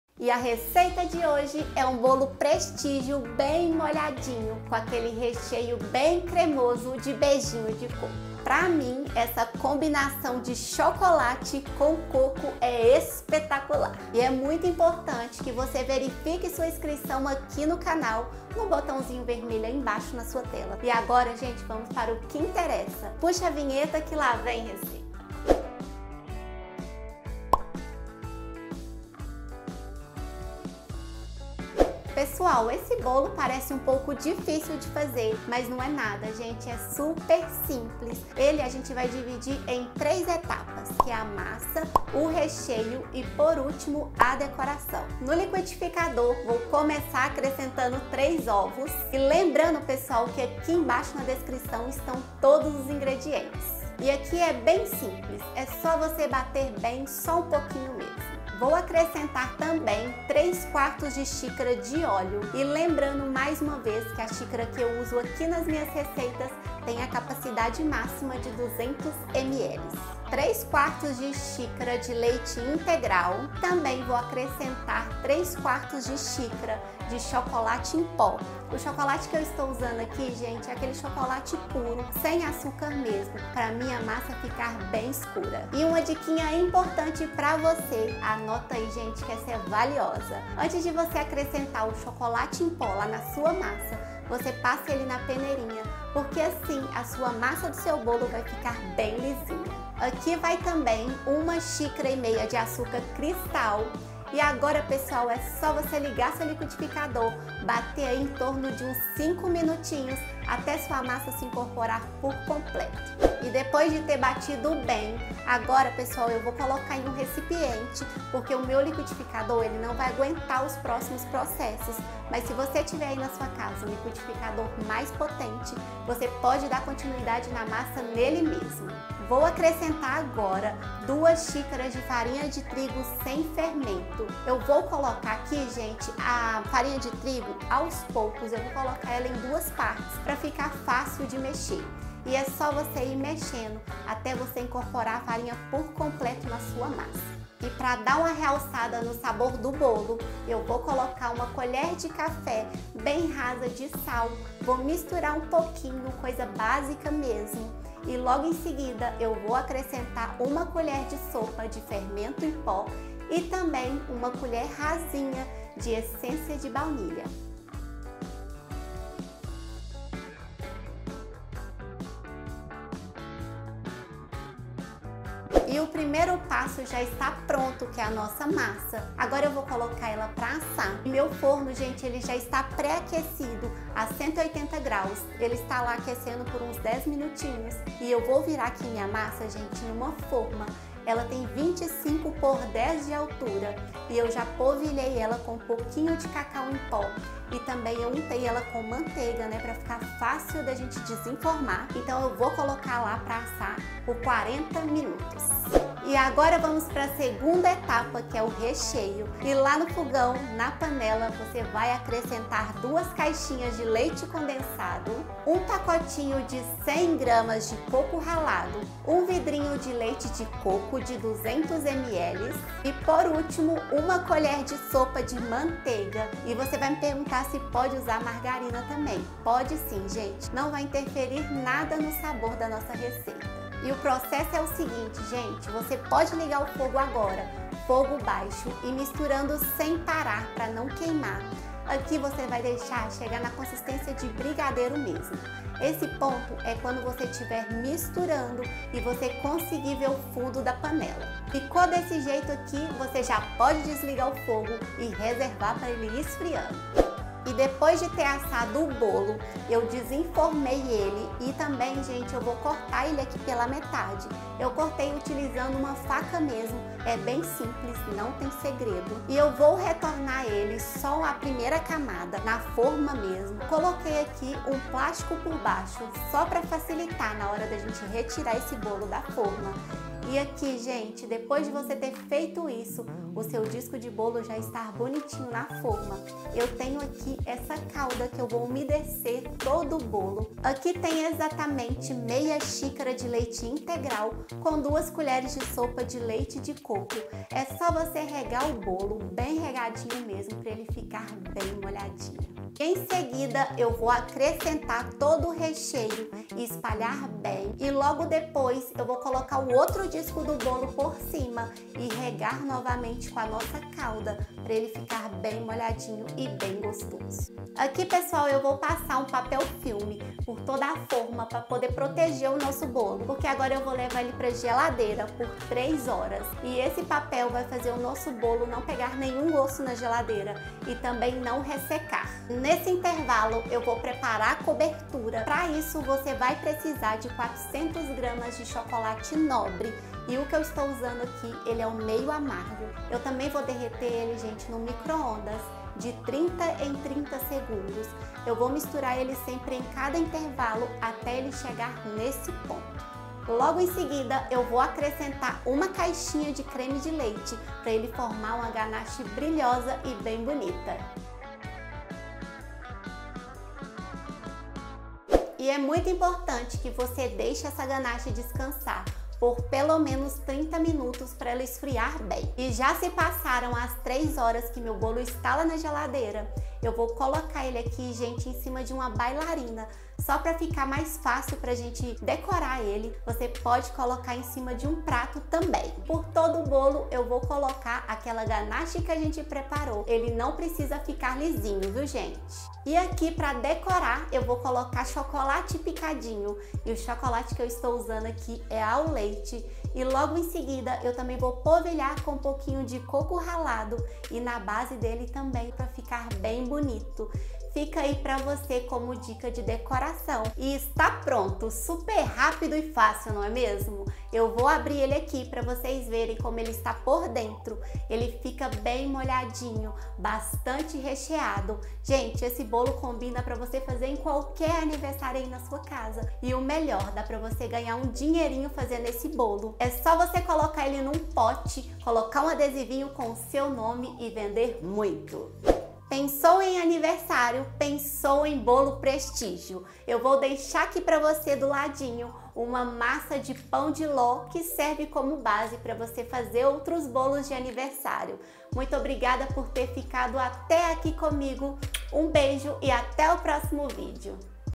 ESSE BOLO DE PRESTÍGIO SUPER RECHEADO VAI SURPREENDER TODOS NA SUA CASA!
Bolo de prestígio recheado

INGREDIENTES:
(MASSA)
3 ovos
3/4 xícara de óleo
3/4 xícara de leite integral
3/4 xícara de chocolate em pó 50% cacau
1 e 1/2 xícara de açúcar
2 xícaras de farinha de trigo
1 colher de café de sal
1 colher de sopa de fermento em pó
1 colher de sopa de essência de baunilha

(RECHEIO)
2 caixinhas de leite condensado
100g coco ralado
200ml leite de coco
1 colher de sopa de manteiga ou margarina

(CALDA)
1/2 xícara de leite integral
2 colheres de sopa de leite de coco

(COBERTURA)
400g chocolate meio amargo
1 caixinha creme de leite

(DECORAÇÃO)
Coco ralado
Chocolate ao leite

OBS: xícara de 200ml  |  forma 25x10 cm